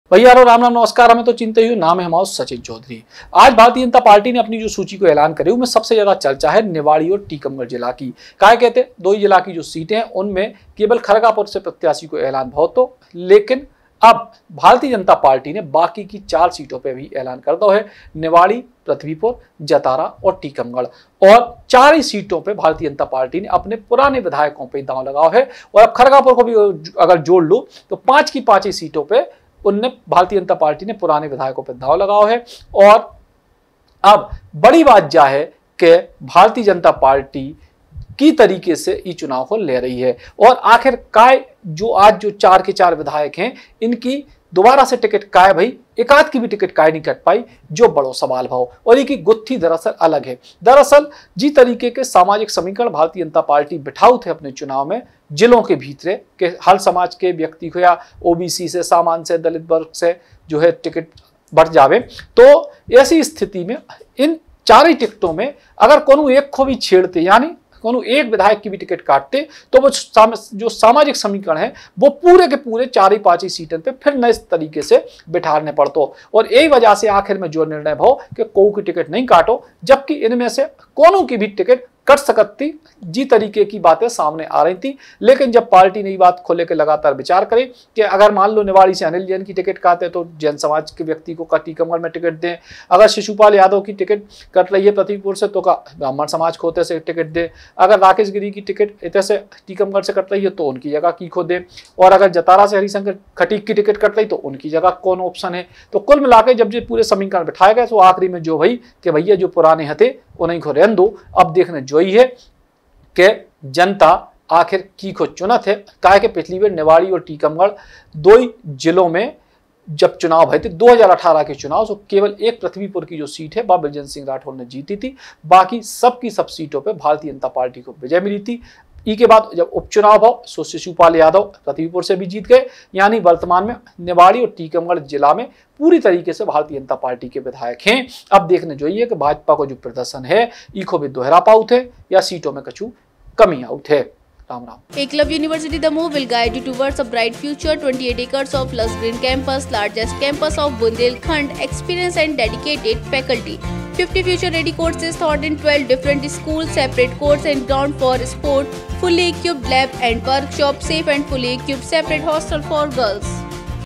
टीकमगढ़ और निवाड़ी जिले में भाजपा ने जताया अपने विधायकों पर विश्वास, अब जनता तय करेगी भविष्य
BUNDELI BAUCHAAR

Suggested Pages :-